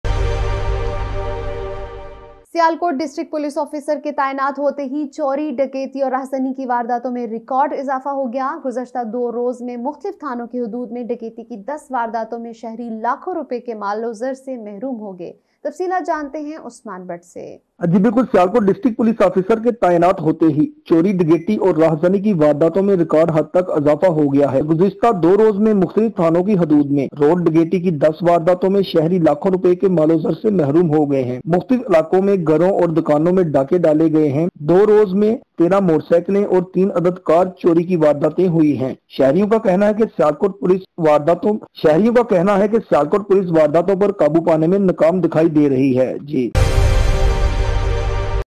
Criminal element│Record addition│Citizens deprived of wealth
Program director : Usman Butt
Light:

As soon as the Sialkot District Police Officer was appointed, there has been a record increase in the incidents of theft, robbery and robbery.